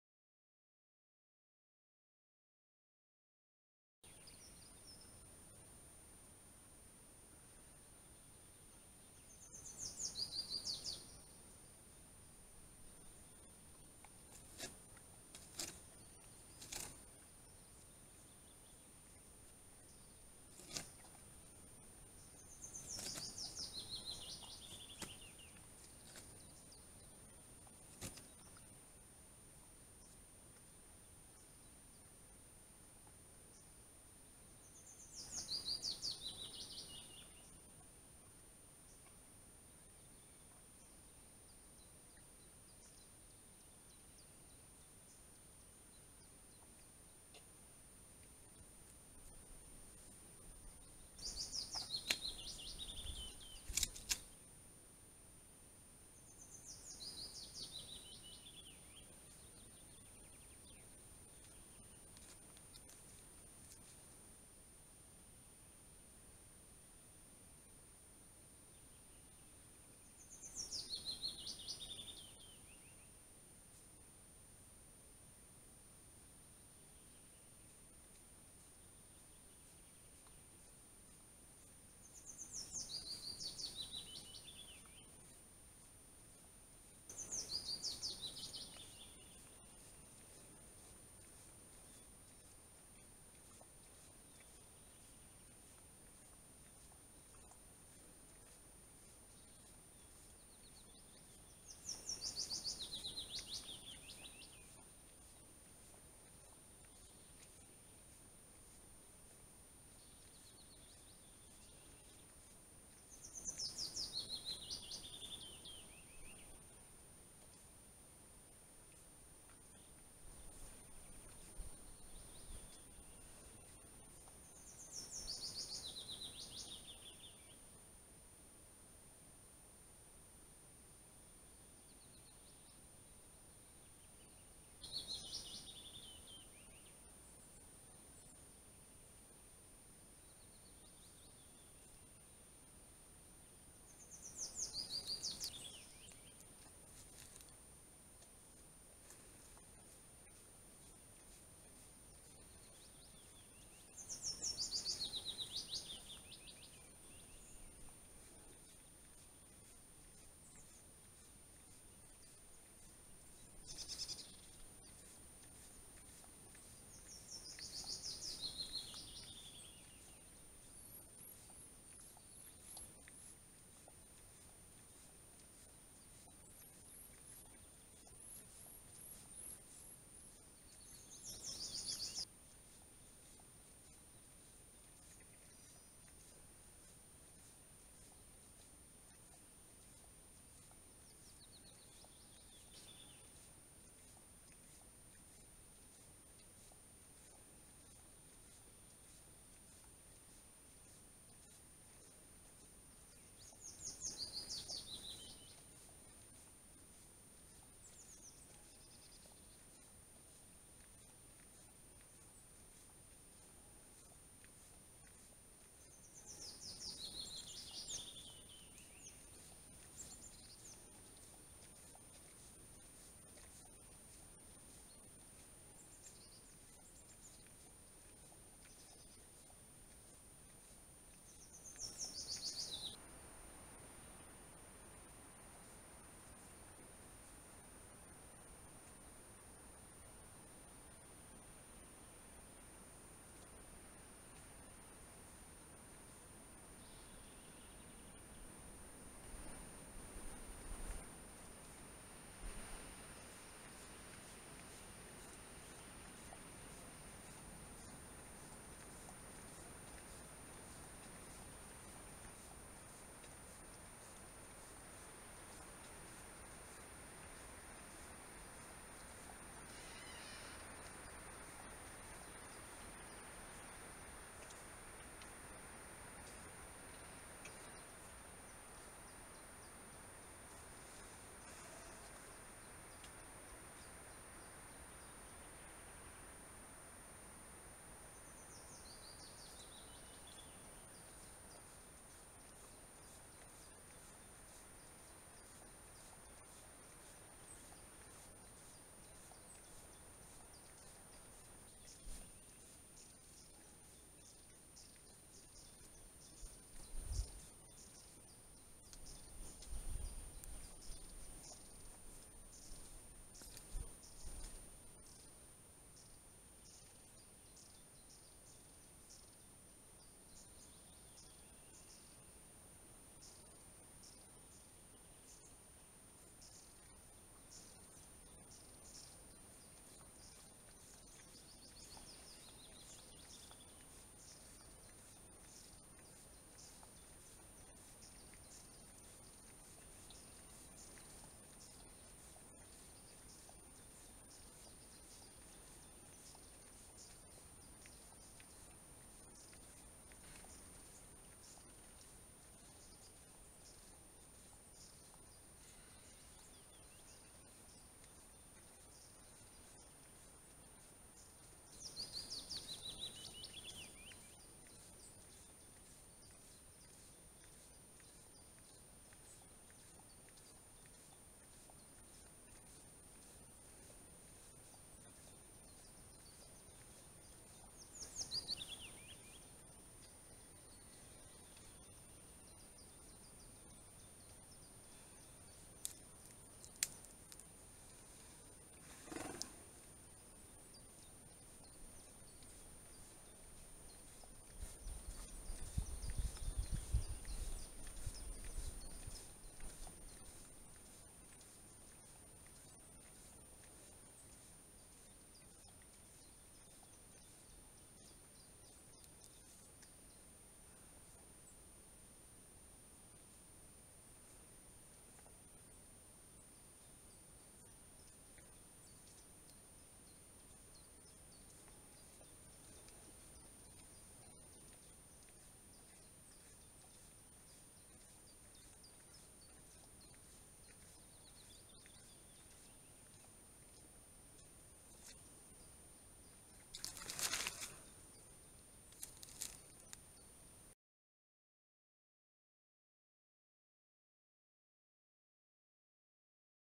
Новый солонец для лося.Фотоохота (часть 2).Лось нашел новый солонец №1
Фотоловушка запечатлела, как рогатый лось пришёл на солонец и смотрит прямо в камеру. Как же хороша природа России весной!

На канале ДикийМир вы найдете эксклюзивные видео животных в их естественной среде обитания и интересные факты о них